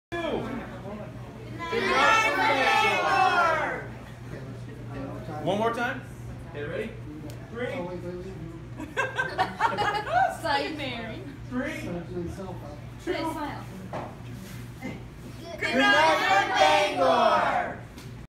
St. Joe's says Good Night on WABI!
During our Lights of Remembrance event last night Brian Sullivan had us say Good Night from Bangor!